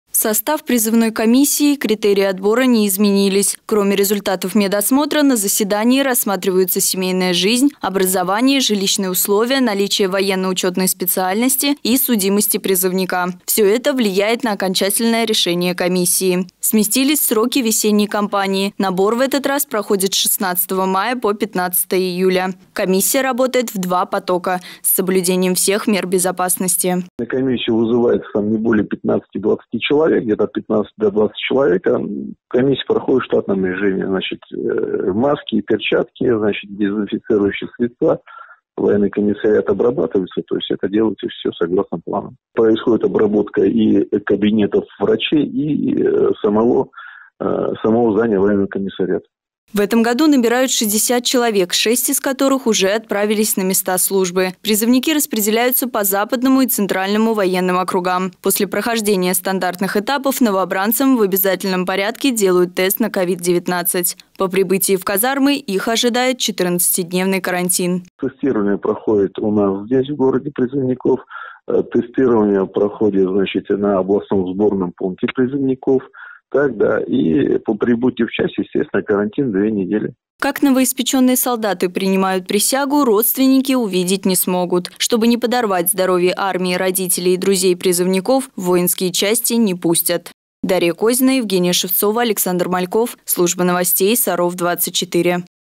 Весенний призыв
Этой весной из-за пандемии коронавируса призывная кампания проходит в условиях повышенных мер безопасности.